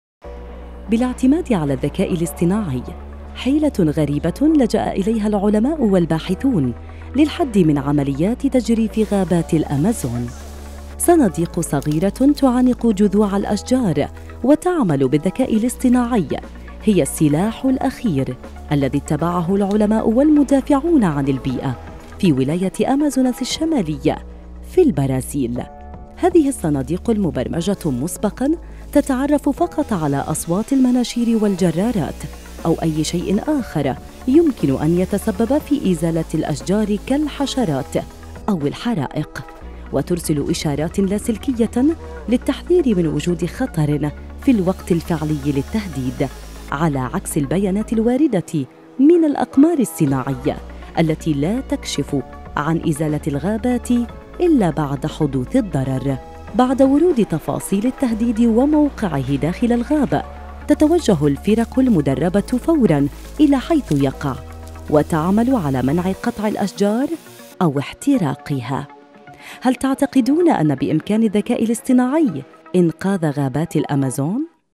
أشجار الأمازون في خطر.. سلاح أخير لجأ إليه العلماء لحماية رئة الكوكب
عبر صناديق صغيرة تعانق جذوع الأشجار.. حيلة غريبة بـ الذكاء_الاصطناعي لجأ إليها العلماء للحد من تجريف غابات
لمعرفة أحدث المستجدات.. عبر البث المباشر من هنا ⤵
وبإمكانكم متابعة العربية على منصات التواصل